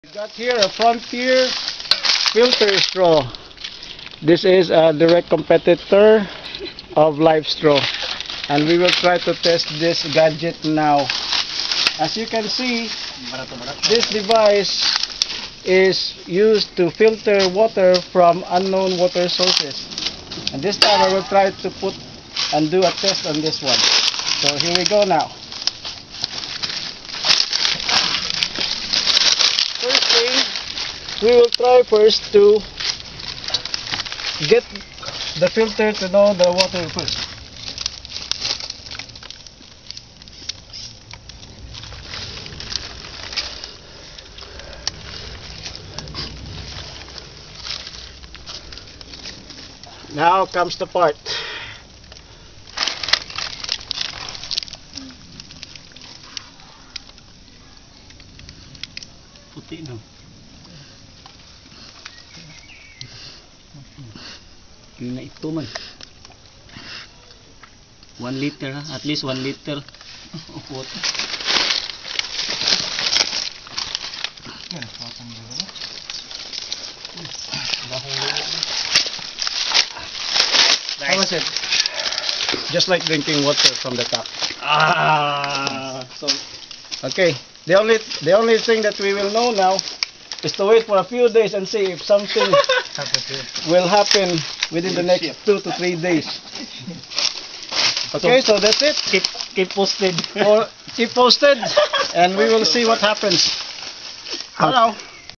Walter Filter Test, 05 May 2013
Guns Pestaño of Camp Red Bushcraft & Survival Guild field tests another brand of a micro water filter set on a water hole of a dried-up mountain stream in a hidden jungle of Cebu City, Philippines.